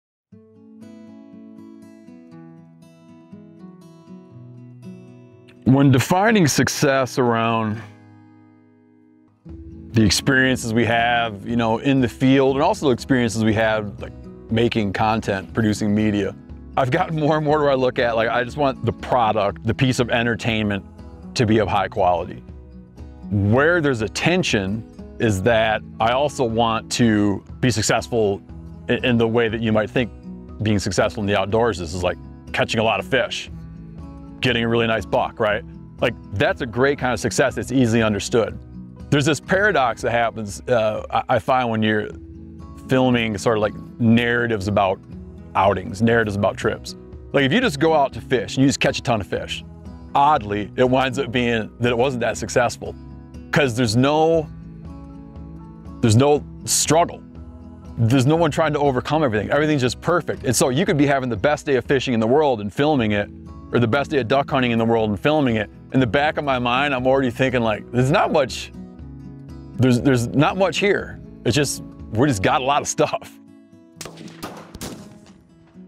Meat Eater Q&A: How Do You Define Success
Meat Eater's Steven Rinella discusses goals and delivering on them in the World today.